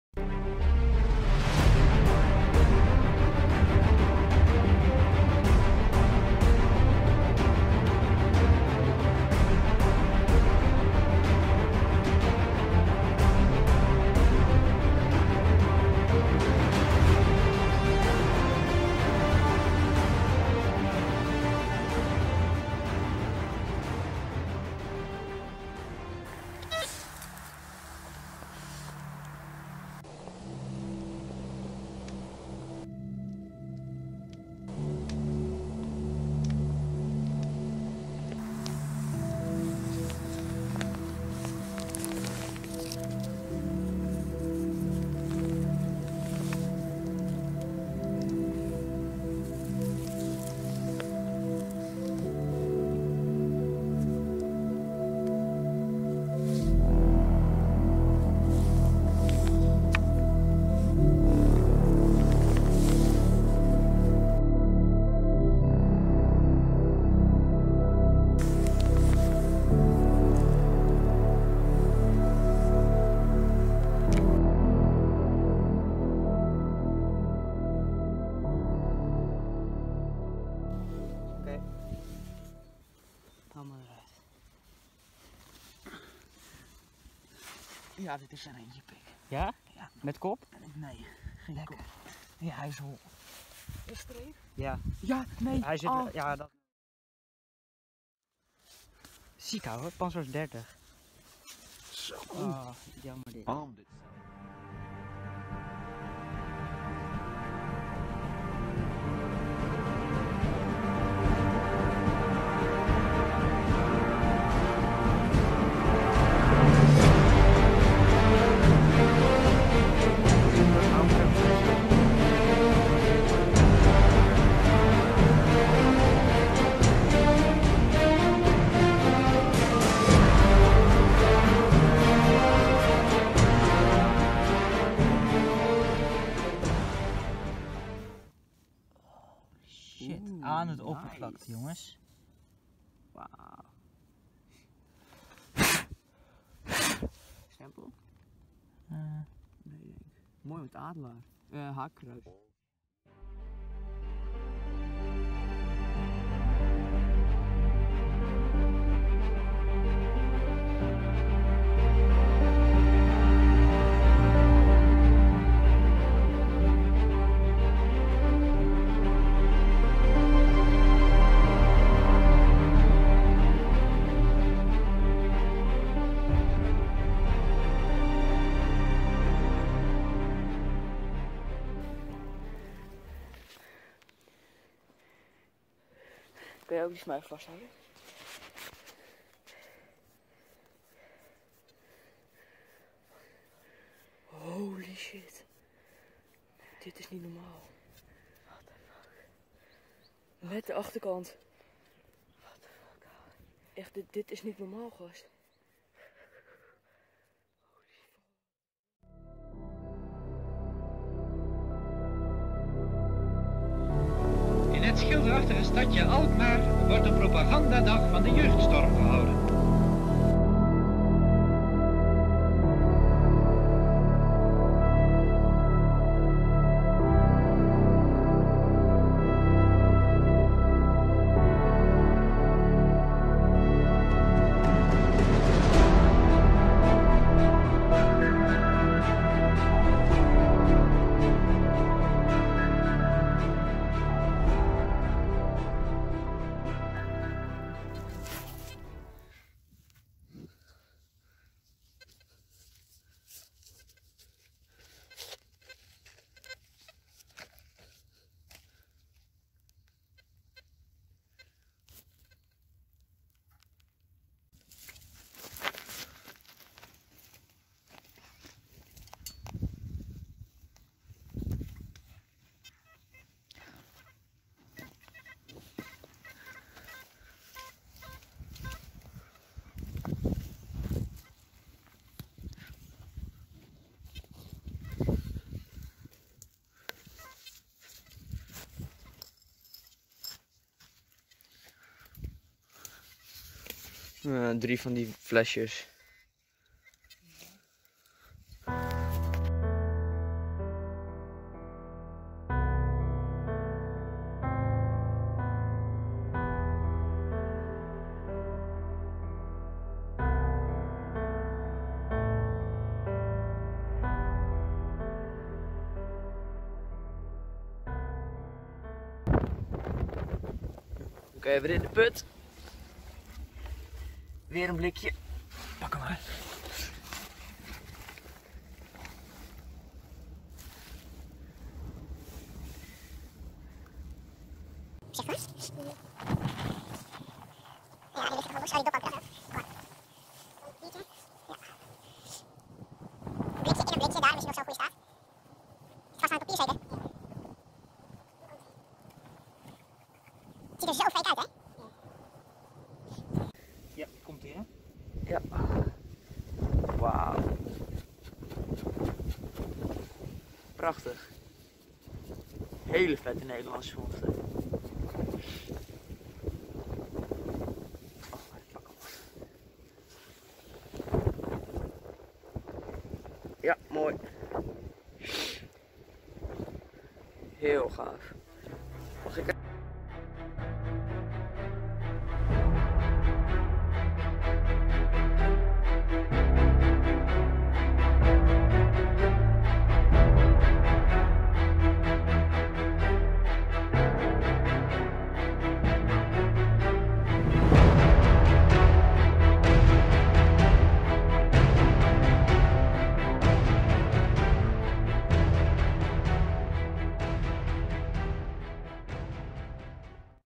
WW2 MetalDetecting | Jeugdstorm Belt buckle, Cap eagle and more!
New video!